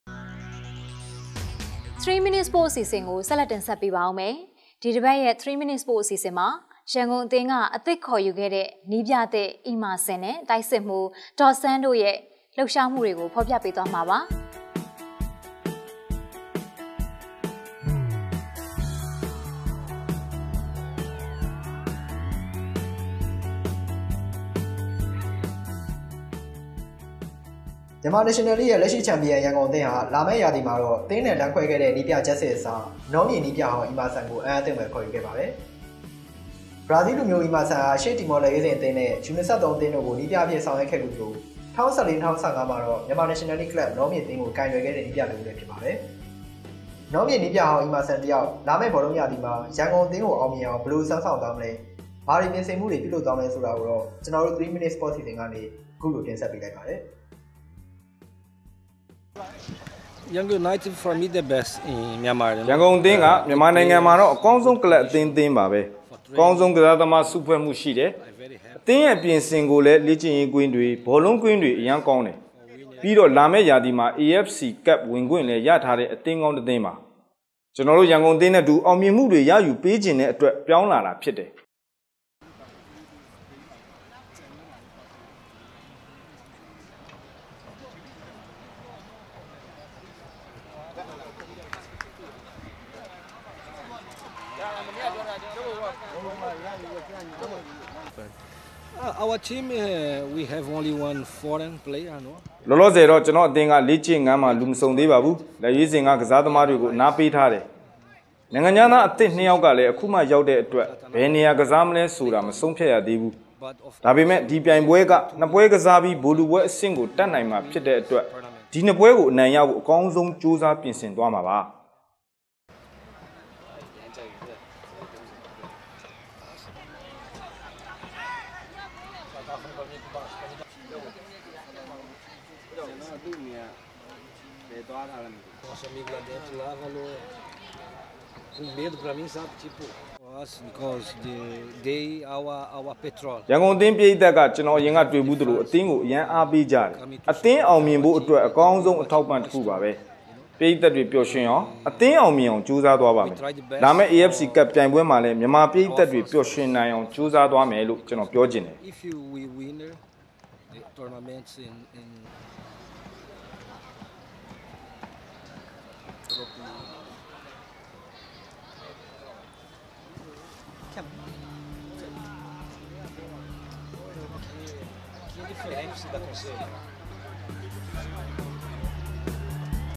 3 minutes sports
အစီအစဥ္အသစ္ျဖစ္တဲ႔ သရီးမိနစ္ အစီအစဥ္ကို တင္ဆက္ေပးပါမယ္။ ဒီတစ္ပတ္ သရီးမိနစ္ စေပါ့စ္မွာ ရန္ကုန္အသင္းက အသစ္ေခၚယူခဲ႔တဲ႔ နည္းျပသစ္ အီမာဆင္နဲ႔ တိုက္စစ္မွဴး ေဒါ့စ္ဆန္းတို႔စ္ တို႔ရဲ႕လႈပ္ရွားမႈေတြကို ေဖာ္ျပေပးသြားမွာပါ။